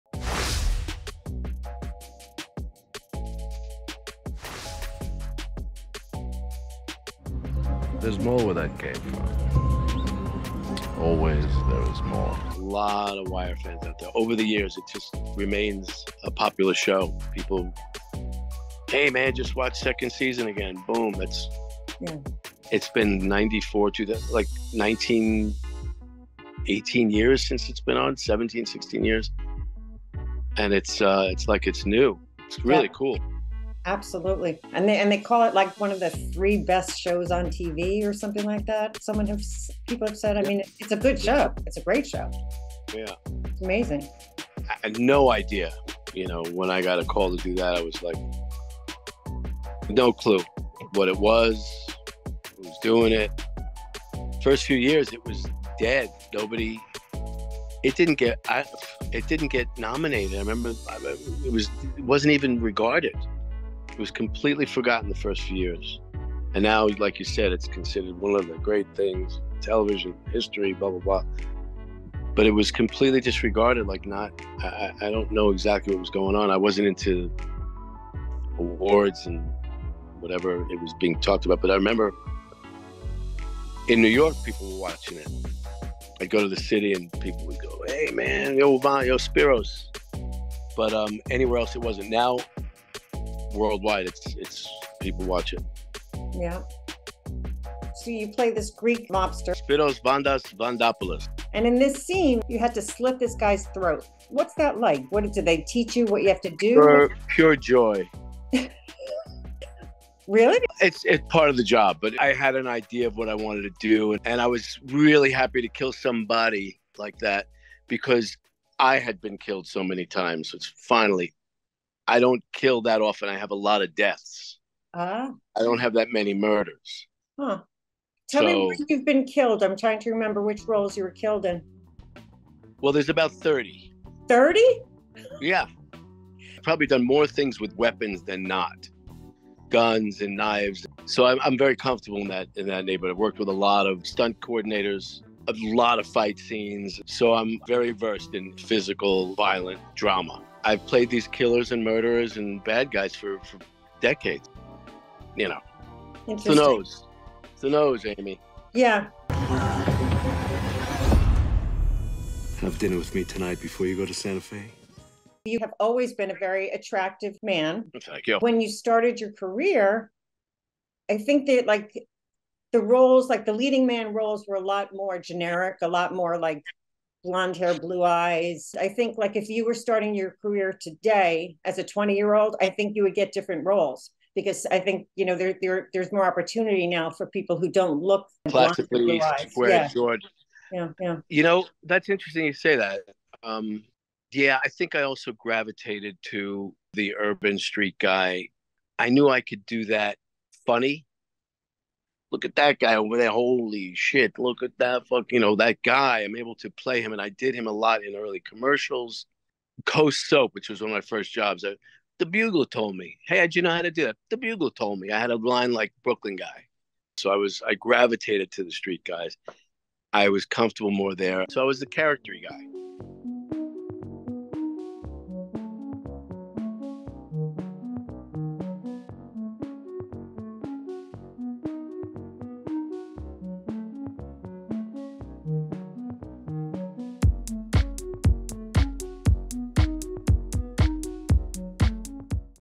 Actor Paul Ben-Victor on The Wire and Why He Plays Bad Guys
Actor Paul Ben-Victor is know for playing Greek mobster Spiro Vondas Vondopoulos on HBO's The Wire and countless other roles on hit TV series and films.

What's the bad guy image all about?

Visit InsideTheShows.com for behind-the-scenes interviews and more, like shopping for exclusive TV-based merch!